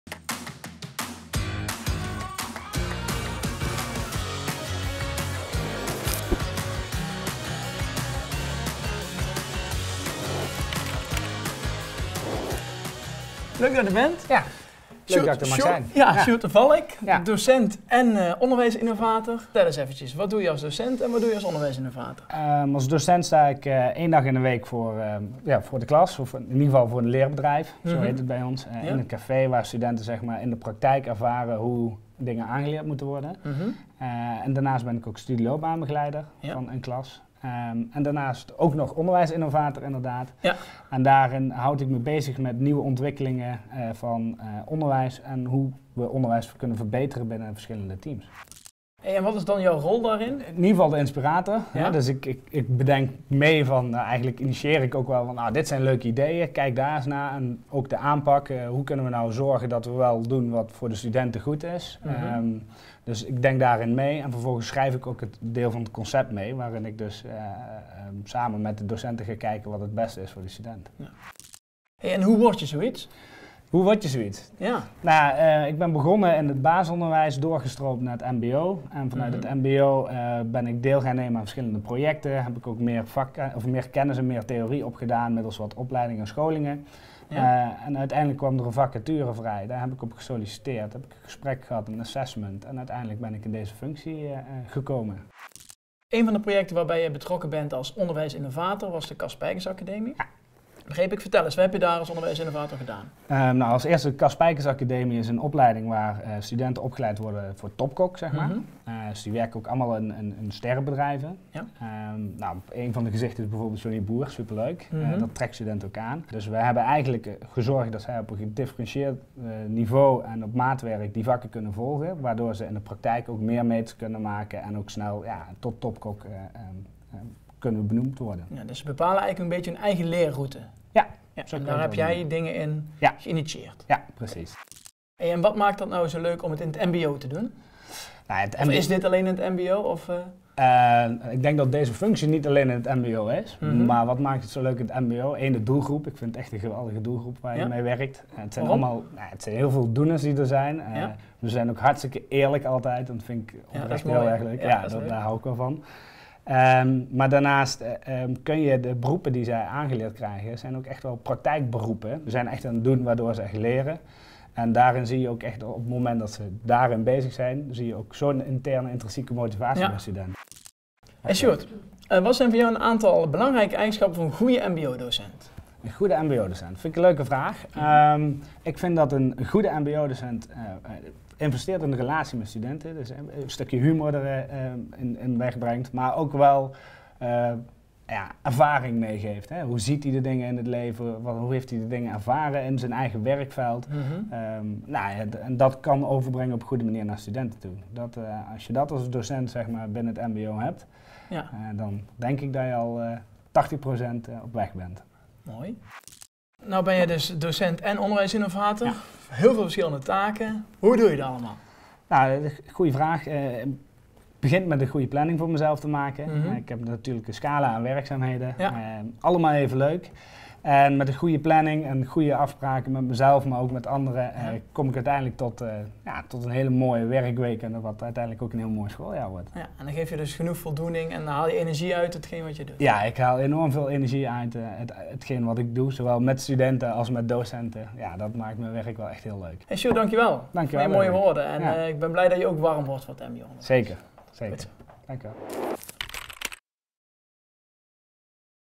Dirk Megens, leraar van het jaar 2019 mbo interviewt collega's
Dirk Megens, leraar van het jaar 2019 mbo, interviewt collega's over hun werk en het mbo. Deze aflevering is Sjoerd de Valk aan het woord, onderwijsinnovator, studieloopbaanbegeleider en docent Nederlands bij ROC Nijmegen.

Sjoerd vertelt onder andere hoe hij in het mbo terecht is gekomen en wat hij als onderwijsinnovator heeft gedaan voor de Cas Spijkers Academie. "Wat zo leuk is aan het mbo? Dat is de doelgroep", zegt Sjoerd.